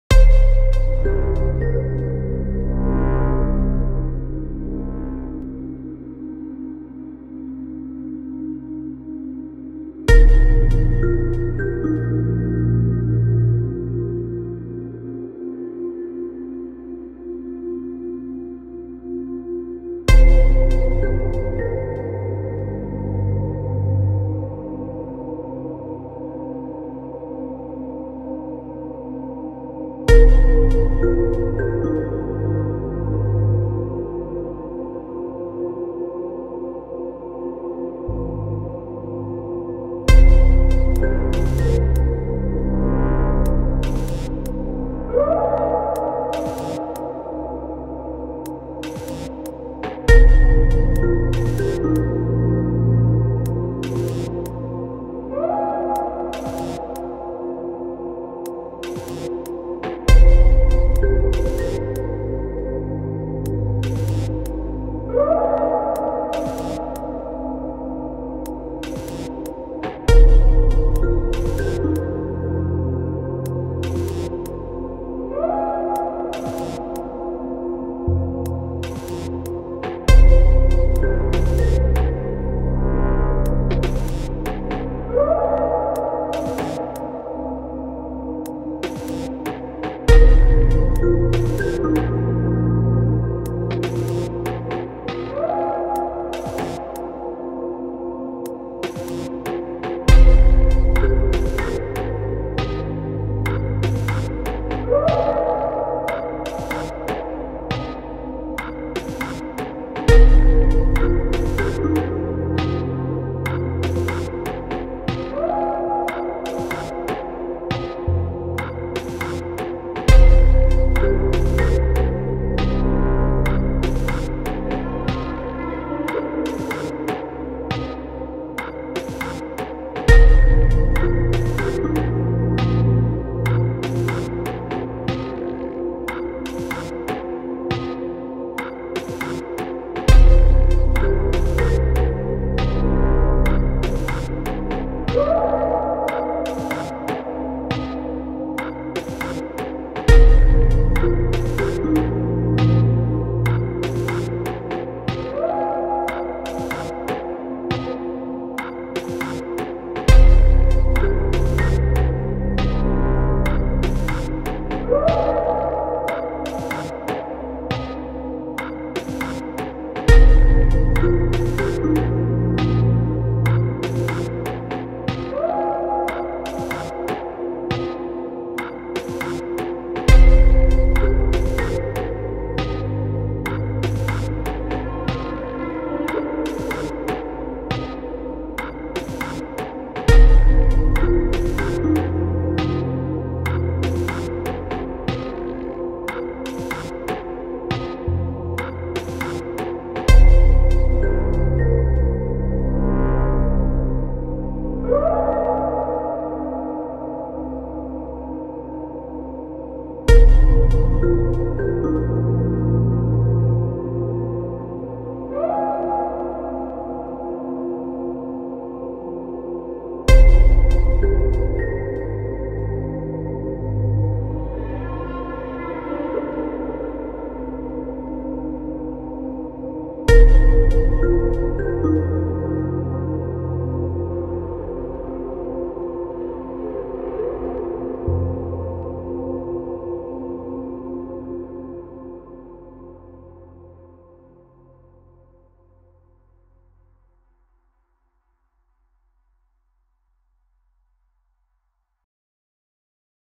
INSTA ART SPEEDPAINT Episode 11 - Sky Whale
Enjoy! New videos daily! I hope you guys like this weeks music, im trying to put more emphasis into the soundtrack depth and quality.